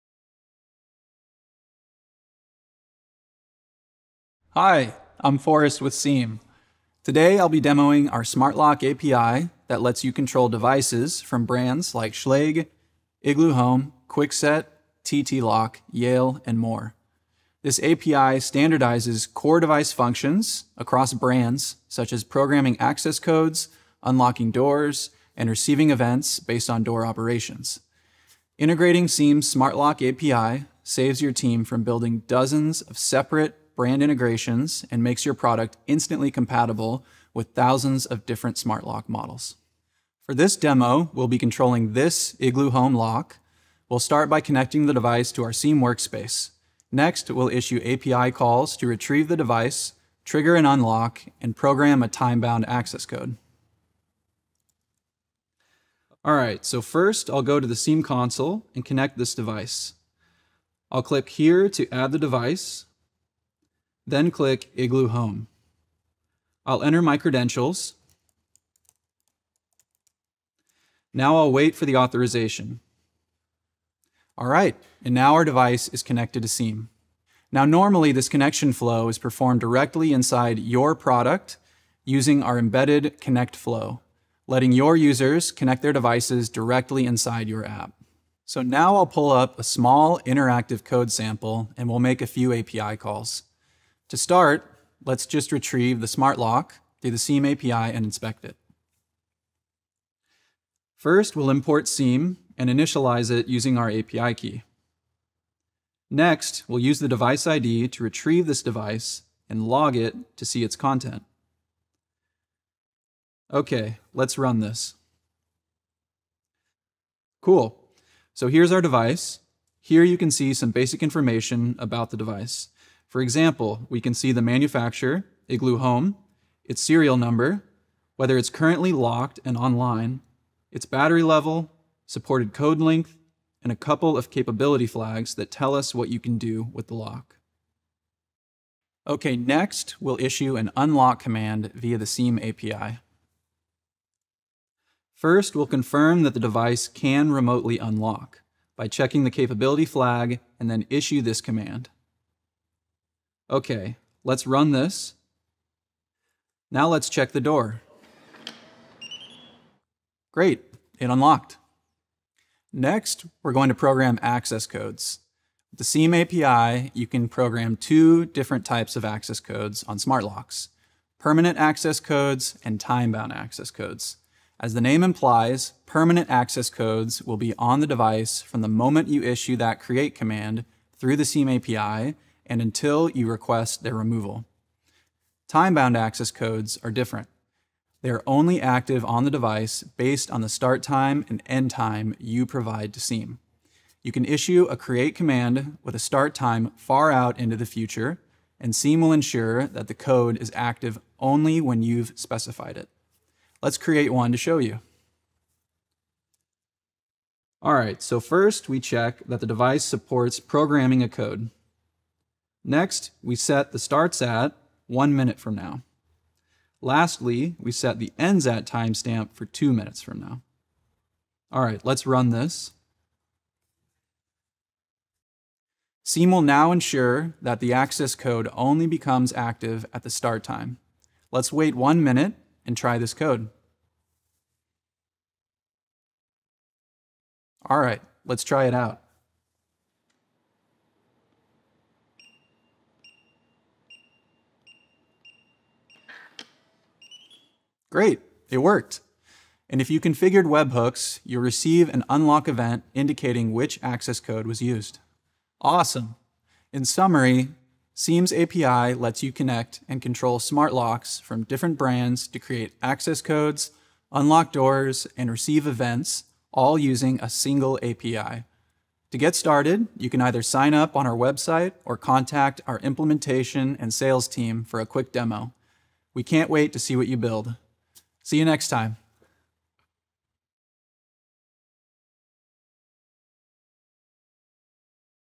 Seam API Demo: Control and Manage Smart Locks (Schlage, Igloohome, Kwikset, TTLock, Yale & More)
In this demo, Forest, Sales Lead at Seam, shows how to use our Smart Lock API to integrate and control locks from brands like Schlage, Igloohome, Kwikset, TTLock, Yale, and more — all through a single, unified API.

💡 Why Seam?
Managing multiple smart lock integrations is complex and time-consuming. Seam standardizes core device functions across brands, so you can unlock doors, program access codes, and receive real-time events without juggling different APIs.

🔑 In this demo you’ll learn how to:
1. Connect a smart lock using Seam Connect
2. Retrieve device details (battery, status, capabilities)
3. Unlock doors remotely via the API
4. Program both permanent and time-bound access codes
5. Receive webhook events when codes are used